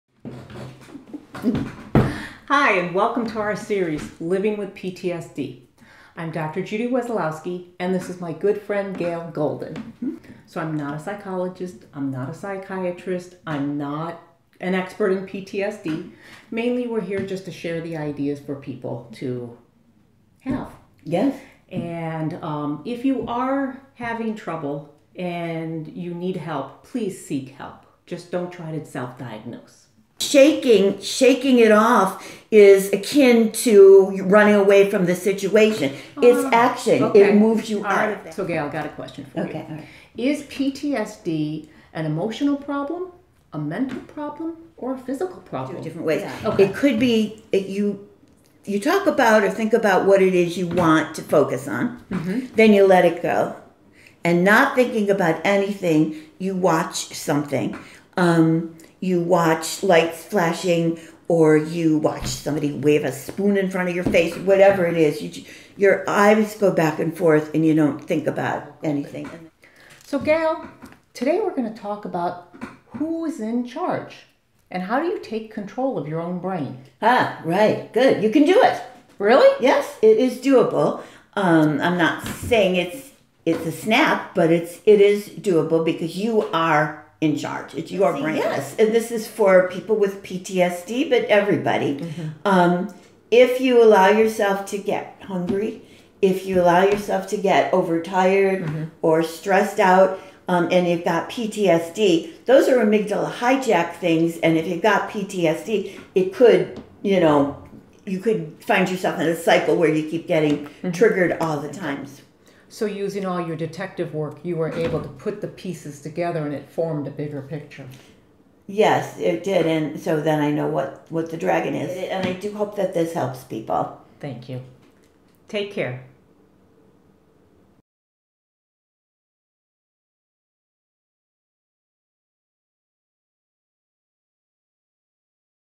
PTSD Trailer
Welcome to our series Living With PTSD.  Dr. Judy Wesolowski, MD and Gail Golden discuss Gail's experiences with PTSD.